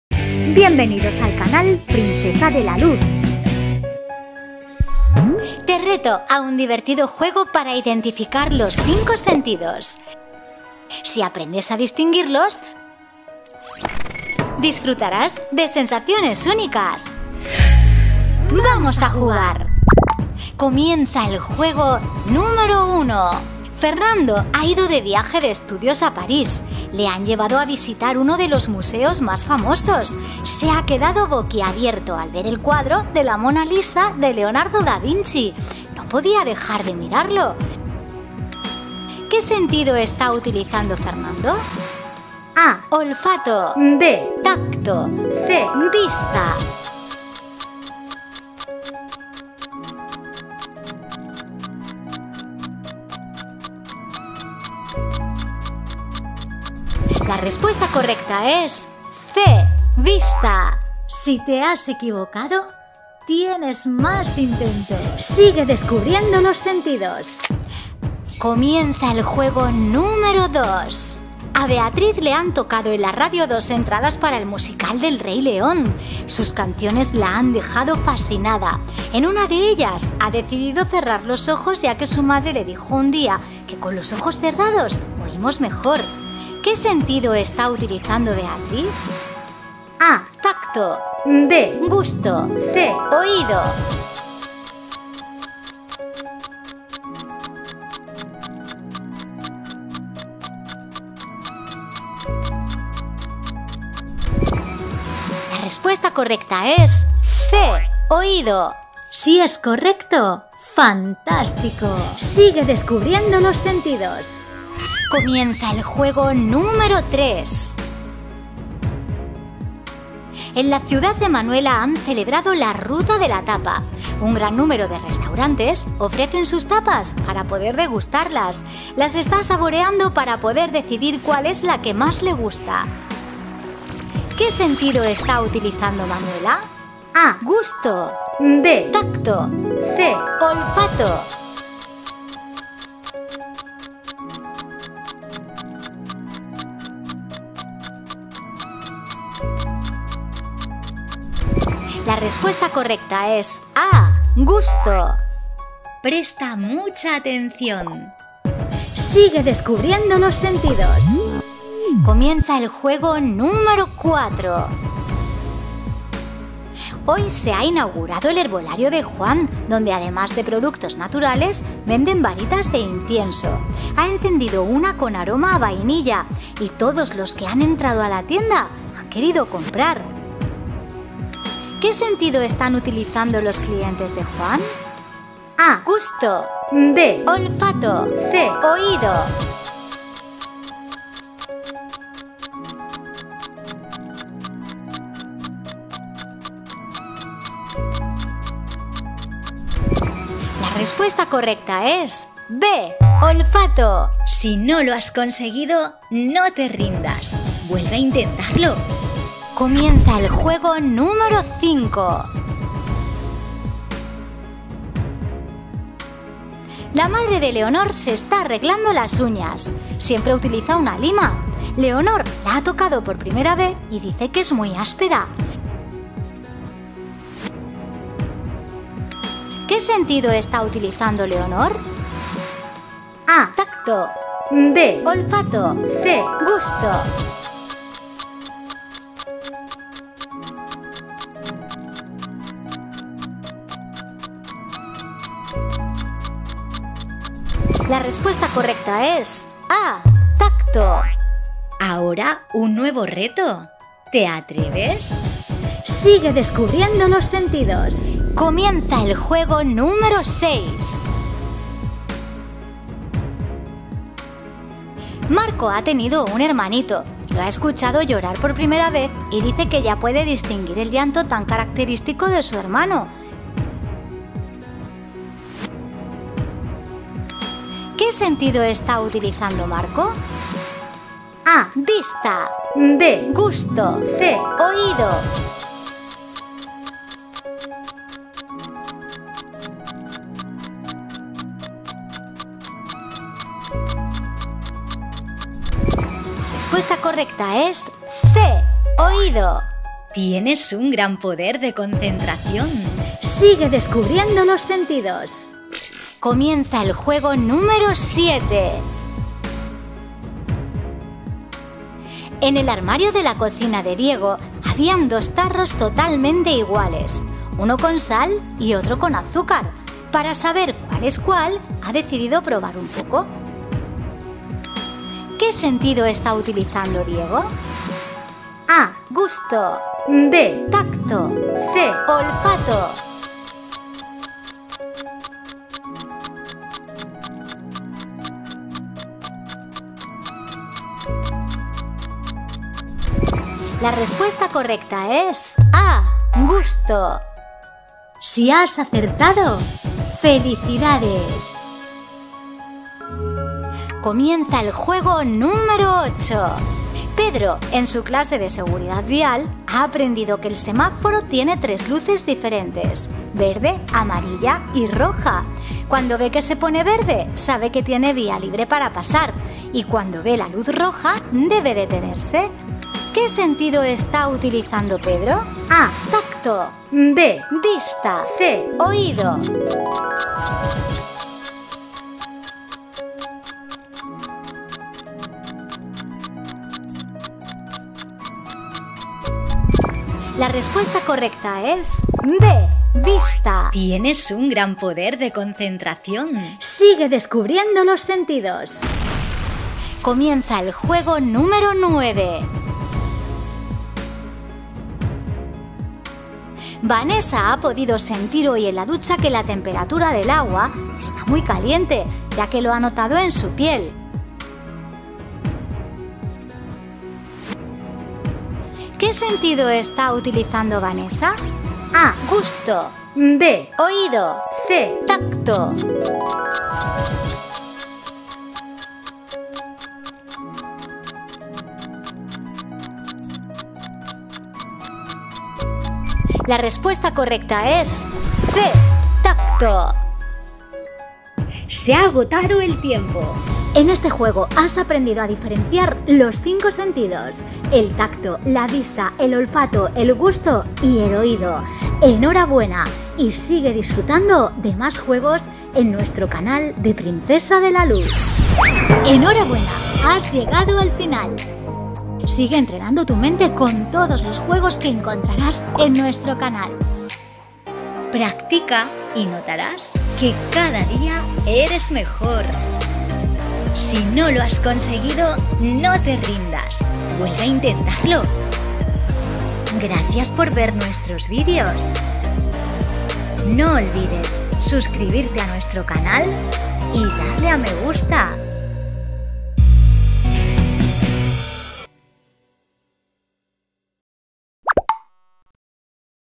LOS 5 SENTIDOS 🌟 Juego En SITUACIONES ▪ VIDEO Educativo Para NIÑOS
Los cinco sentidos, el tacto, el gusto, el oído, el olfato y la vista, nos da la oportunidad de ver, oír, sentir, oler, saborear, todo lo que nos rodea.
En este divertido juego les mostramos situaciones y tendrán que adivinar que sentido están utilizando nuestros protagonistas.

Una manera muy divertida de enseñarlos a reconocerlos.
Un vídeo educativo con el que aprenderán.
Espero que os sirva este material, dirigido a niños de infantil y primaria.